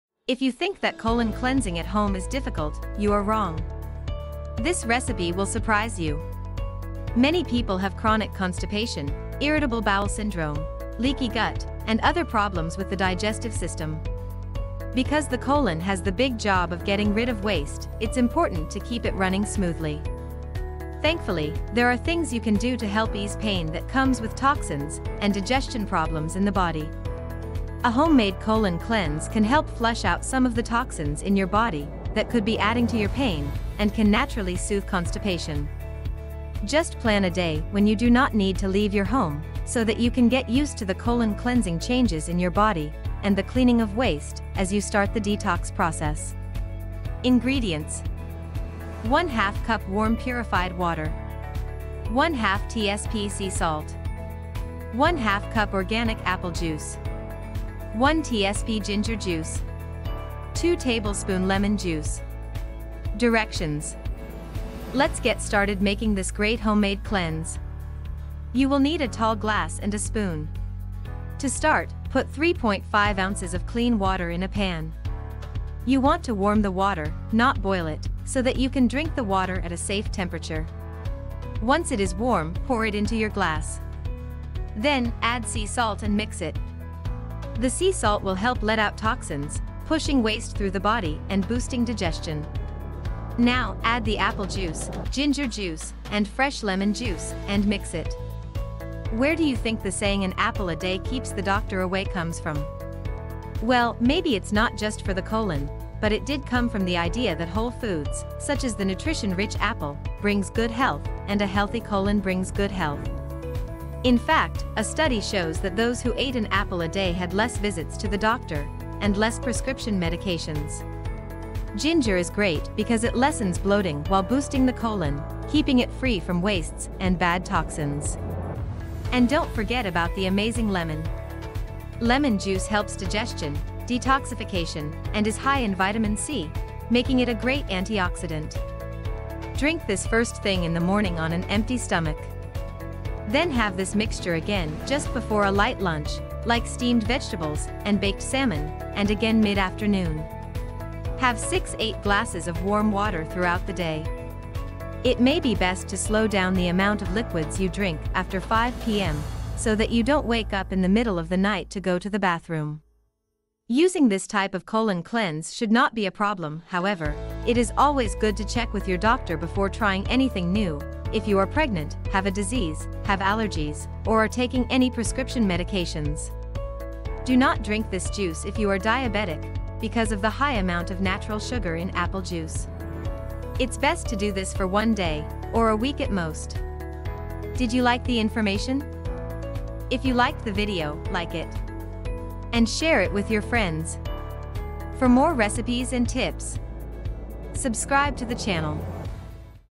Homemade colon cleanse with apple, ginger and lemon juice
Many people have chronic constipation, irritable bowel syndrome, leaky gut, and other problems with the digestive system. Because the colon has the big job of getting rid of waste, it’s important to keep it running smoothly.

Thankfully, there are things you can do to help ease pain that comes with toxins and digestion problems in the body.

A homemade colon cleanse can help flush out some of the toxins in your body that could be adding to your pain, and can naturally soothe constipation. Just plan a day when you do not need to leave your home so that you can get used to the colon cleansing changes in your body and the cleaning of waste, as you start the detox process.

INGREDIENTS:

- ½ cup warm purified water
- ½ tsp sea salt
- ½ cup organic apple juice
- 1 tsp ginger juice
- 2 tbsp lemon juice

DIRECTIONS:

Let’s get started making this great homemade cleanse. You will need a tall glass and a spoon. To start, put 3.5 ounces of clean water in a pan. You want to warm the water, not boil it, so that you can drink the water at a safe temperature.

Once it is warm, pour it into your glass. Then, add sea salt and mix it. The sea salt will help let out toxins, pushing waste through the body and boosting digestion.

Now, add the apple juice, ginger juice, and fresh lemon juice, and mix it.

Where do you think the saying ‘an apple a day keeps the doctor away’ comes from? Well, maybe it’s not just for the colon, but it did come from the idea that whole foods, such as the nutrition- rich apple, brings good health, and a healthy colon brings good health.

In fact, a study shows that those who ate an apple a day had less visits to the doctor, and less prescription medications.

Ginger is great because it lessens bloating while boosting the colon, keeping it free from wastes and bad toxins. And don’t forget about the amazing lemon! Lemon juice helps digestion, detoxification, and is high in vitamin C, making it a great antioxidant.

Drink this first thing in the morning on an empty stomach. Then have this mixture again just before a light lunch, like steamed vegetables and baked salmon, and again mid- afternoon.
Have 6- 8 glasses of warm water throughout the day. It may be best to slow down the amount of liquids you drink after 5 p.m. so that you don’t wake up in the middle of the night to go to the bathroom.

Using this type of colon cleanse should not be a problem, however, it is always good to check with your doctor before trying anything new, if you are pregnant, have a disease, have allergies, or are taking any prescription medications.

Do not drink this juice if you are diabetic because of the high amount of natural sugar in apple juice.

❤️ Natural Cures brings health information and tips for people looking natural ways to prevent, treat and heal ailments.